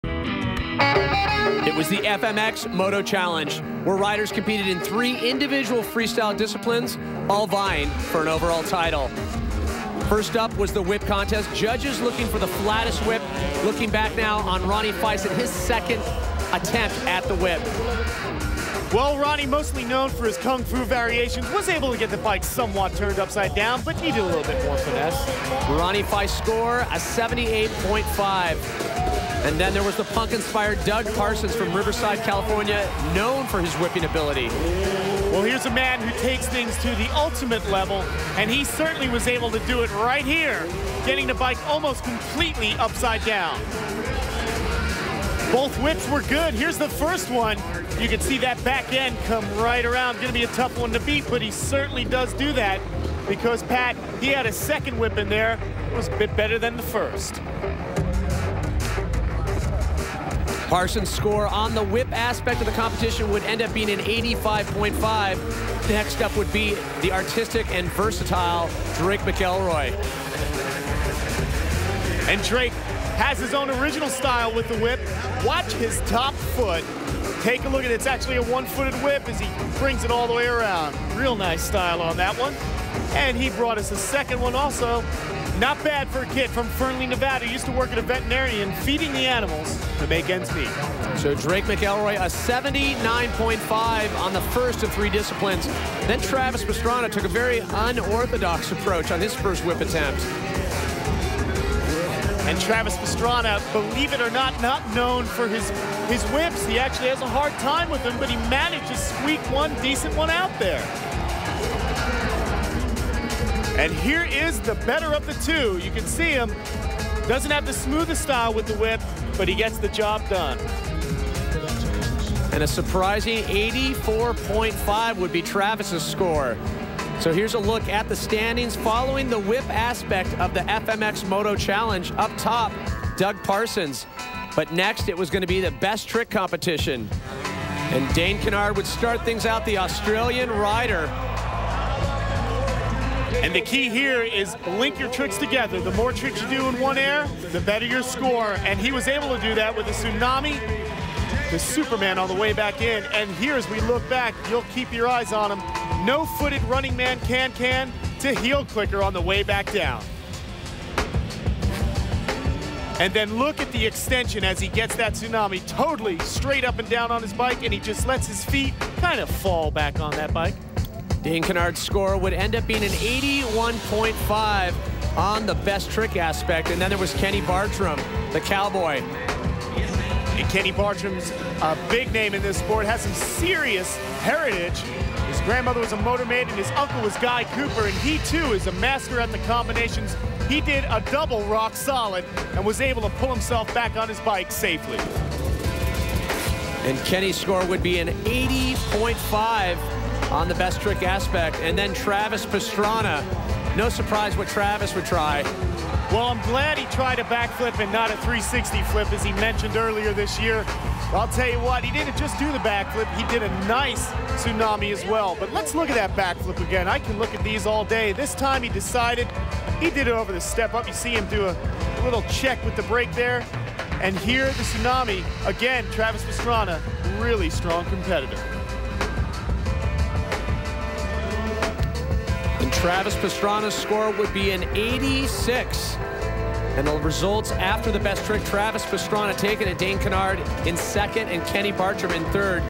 Gravity Games 2002
Some highlights of best whip and best trick from 2002 Gravity Games. Unfortunately i don´t have the full contest but i have from Freestyle some footage and i will upload soon. Enjoy